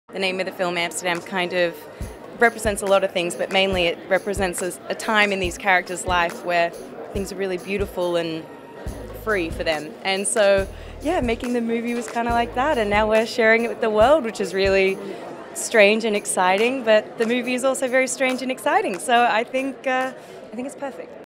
MARGO 'IT'S STRANGE AND EXCITING' AMSTERDAM!
Set in the '30s, it follows three friends who witness a murder, become suspects themselves, and uncover one of the most outrageous plots in American history.

Starring margotrobbie ChistianBale RamiMalek johndavidwashington and robertdeniro in this new movie! By the way Drake has executively produced it! Amsterdam is a period mystery comedy film directed, produced and written by David O. Russell. Look out for TaylorSwift who pops up!

AMSTERDAM is out October 7, 2022